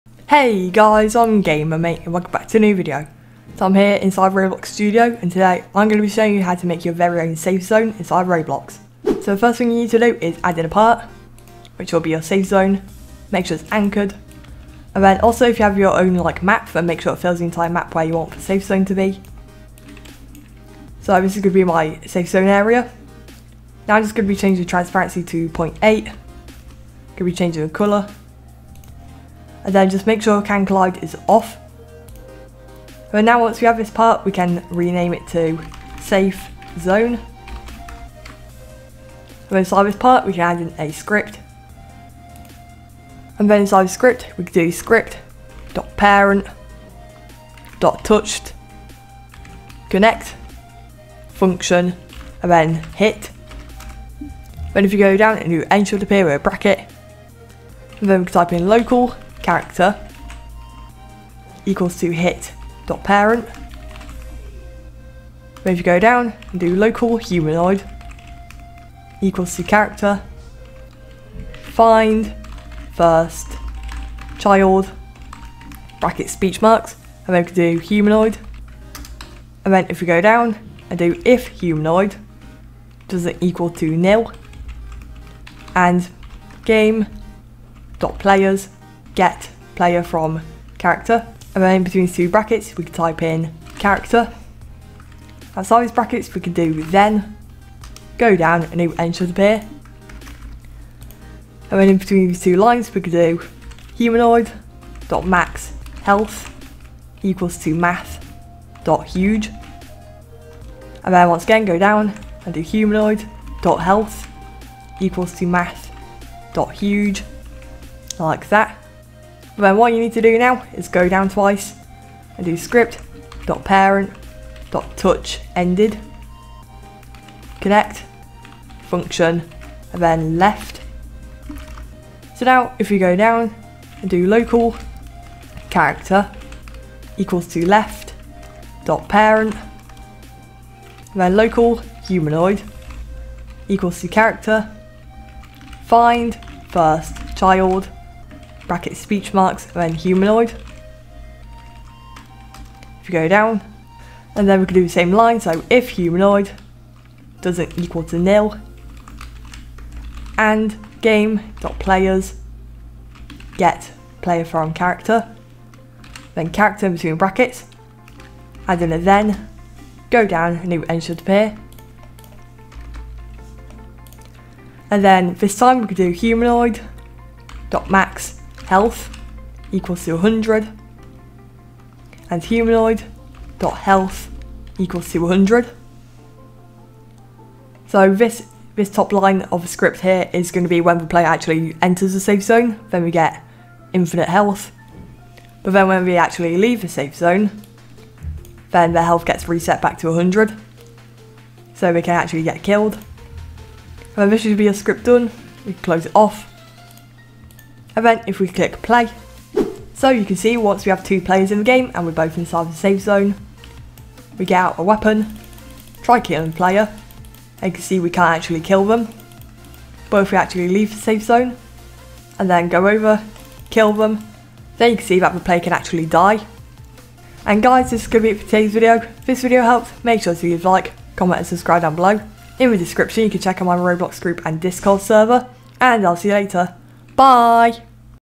ROBLOX Studio Safe Zone Tutorial
Today I'm showing you how to make your own working safe zone area in ROBLOX Studio